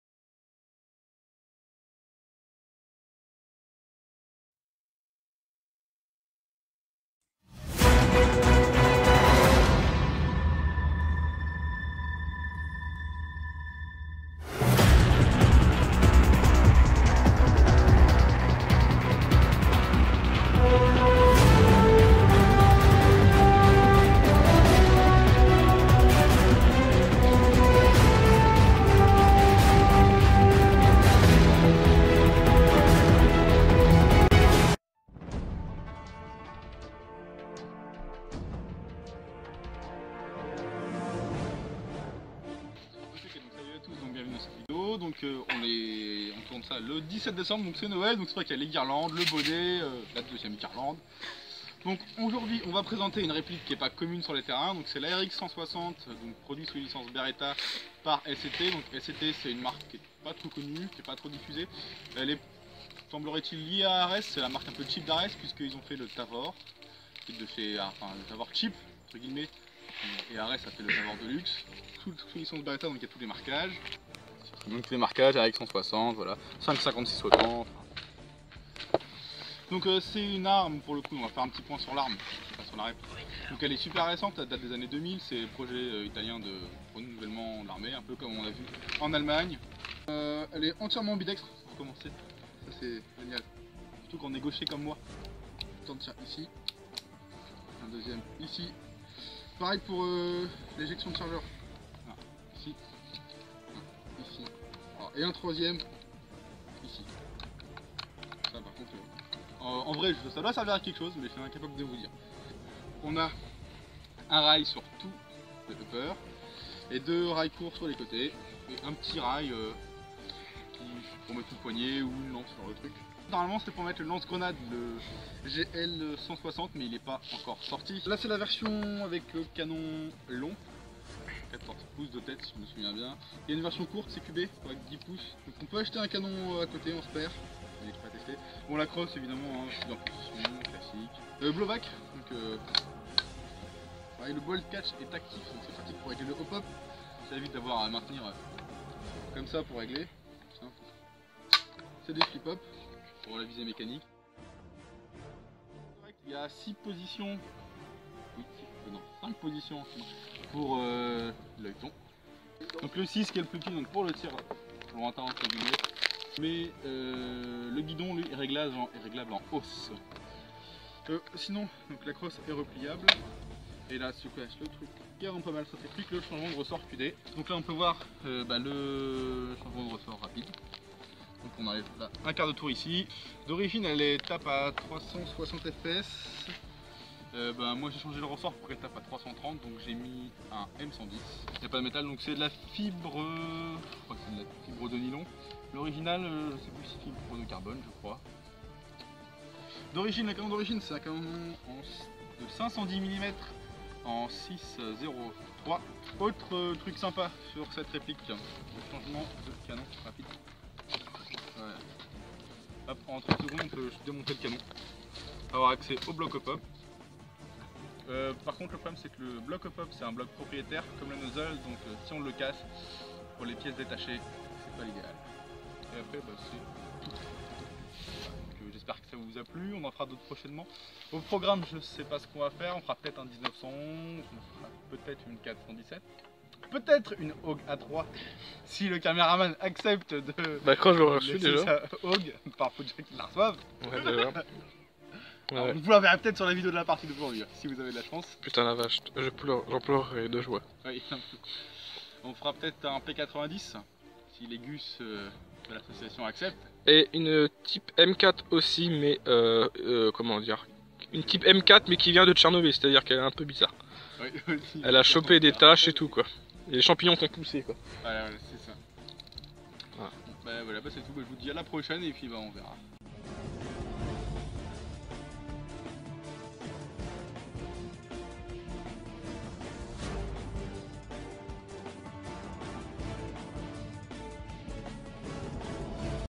Présentation Sommaire #2 ARX 160 Association d'Airsoft de la Baie du Mont Saint Michel
Second épisode de présentation sommaire de quelques répliques de l'association d'airsoft de la Baie du Mont Saint Michel